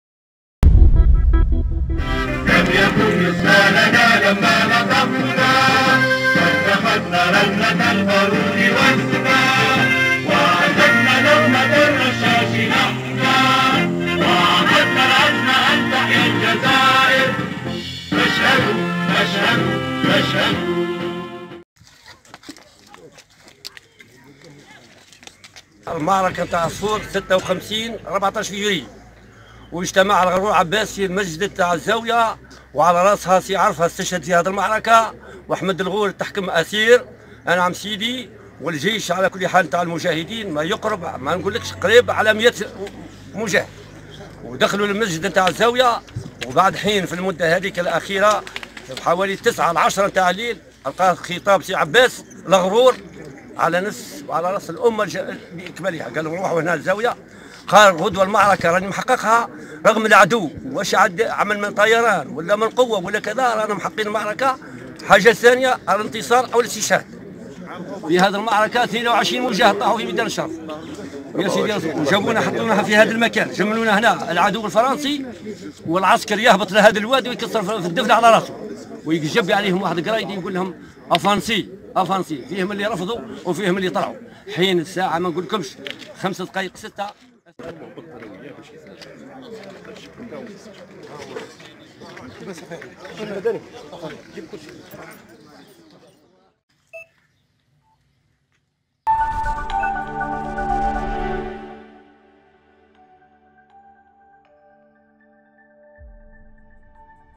خنشلة: قرية الزاوية بجبل ششار تحيي الذكرى الـ64 لمعركة العصفور
خنشلة (وأج)- قرية الزاوية بجبل ششار تحيي الذكرى الـ64 لمعركة العصفور.